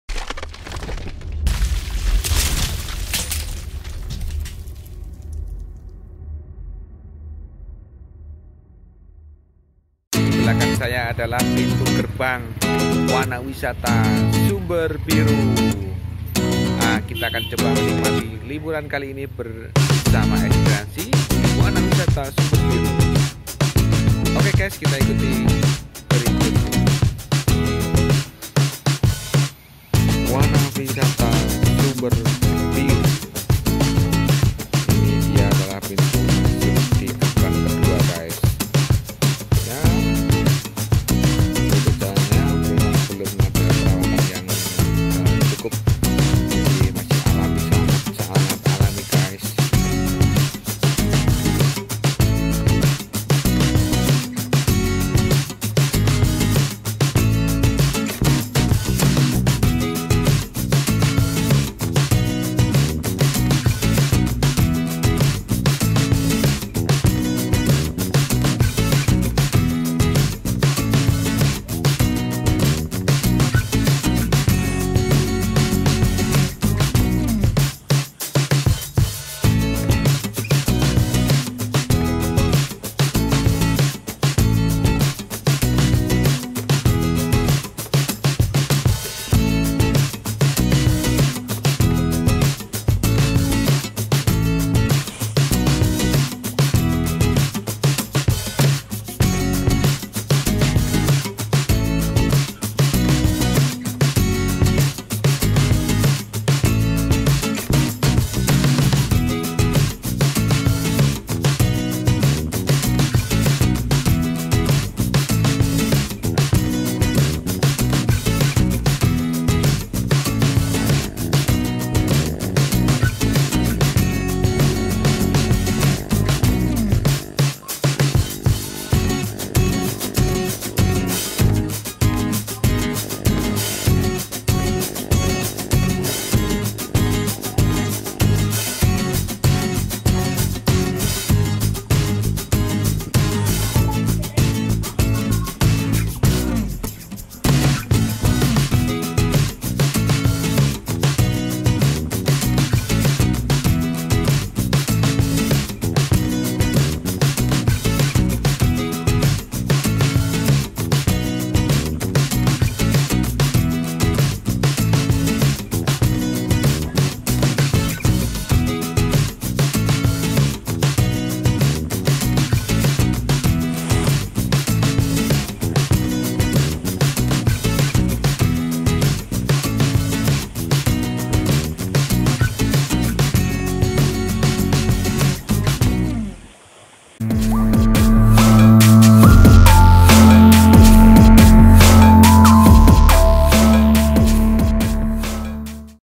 Wisata Sumber Biru Icon Kota Jombang
wana Wisata Sumber Biru
berada di kecamatan Wonosalam Kabupaten Jombang.

Wisata Sumber Biru adalah wisata buatan yang bernuansa alam karna lokasi banyu biru terletak di lereng pegunungan, suasana yang sejuk dan asri akan membuat siapa saja betah dan berlama- lama berada di wisata Sumber Biru

Lokasi wisata Sumber Biru berada di lereng pegunungan, tepatnya di Dusun Ngembak, Desa Wonomerto, Kecamatan Wonosalam yang memang tempatnya agak jauh dari perkotaan, tapi kalau kita kita sudah sampai di wisata Sumber Biru seolah rasa capek kita akan terbayar oleh pesona nuansa alamnya yang asri. map lokasi

Harga tiket masuk wisata Banyu Biru relatif sangat murah karna kita tidak perlu merogoh kocek yang dalam, Untuk bisa masuk ke tempat wisata kita hanya perlu membayar Rp5000/orang itu juga sudah termasuk parkir kendaraan.